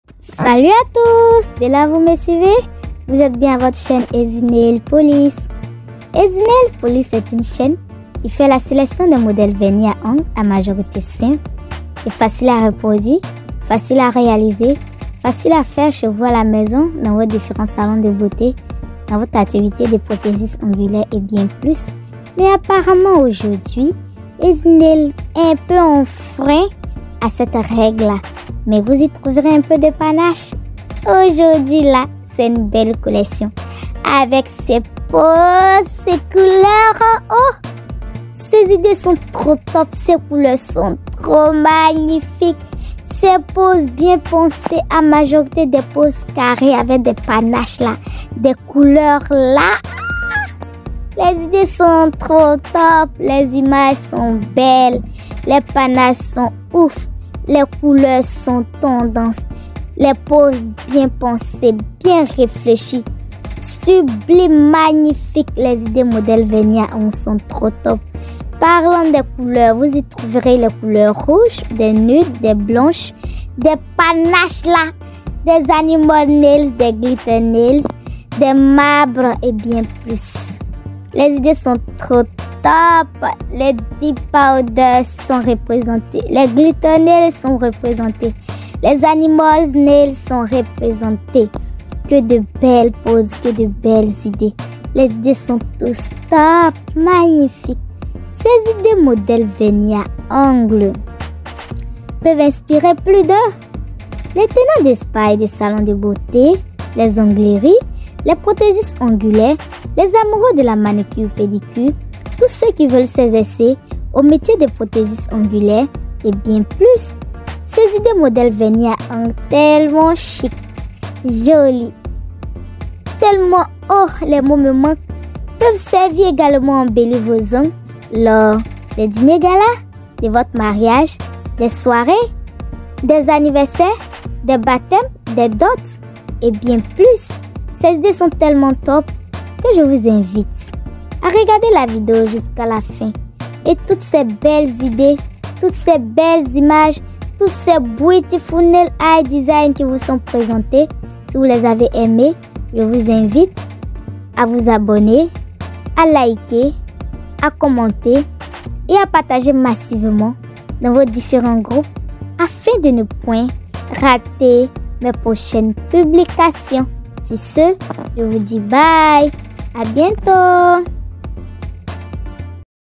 top collection Modèles d'ongle d'art fashion nail art designs collection tendency nail polish
salut belle audience, j'ai sélectionné pour vous de belles idées modèles vernis à ongles"nail art designs " simple et facile à reproduire. Vous  y trouverez aussi d'autre designs mais  l'accent est majoritairement mis sur les modèles d'ongles d'art simple à réaliser.

bon visionnage à vous!!! @Easynailpolish